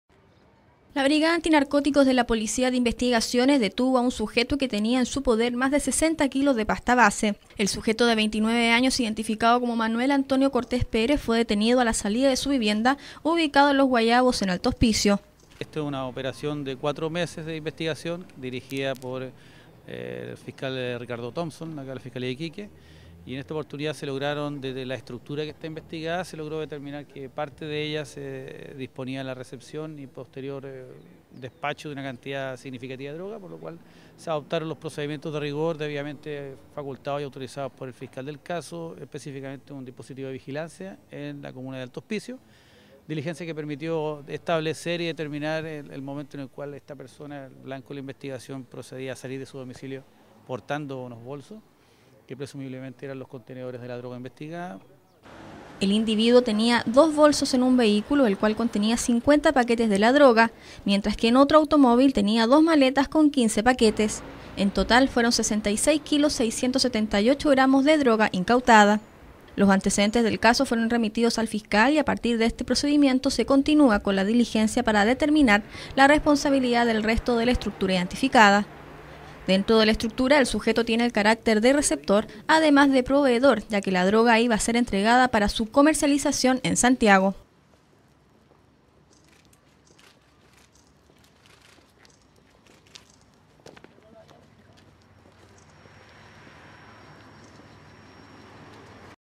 DECOMISAN MÁS DE 60 KILOS DE PASTA BASE DE COCAÍNA
La brigada anti narcóticos de la policía de investigación detuvo a un sujeto que tenía en su poder más 60 kilos de pasta base.